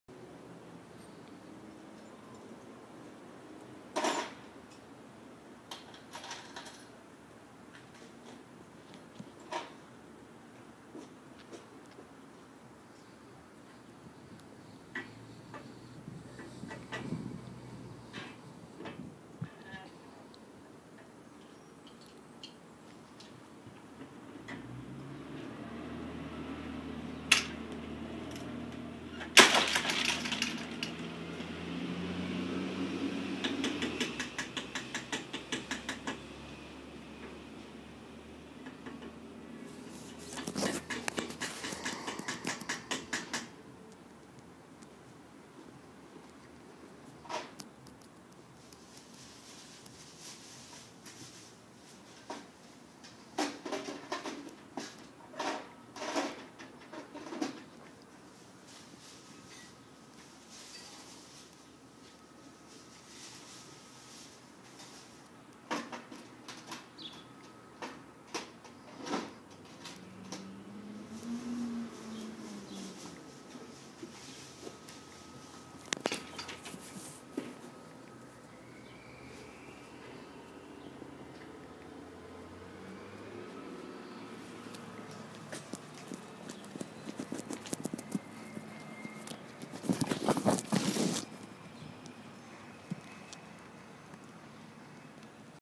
Josh landlord 260 john garland blvd
trespassing as 24 hr written notice not provided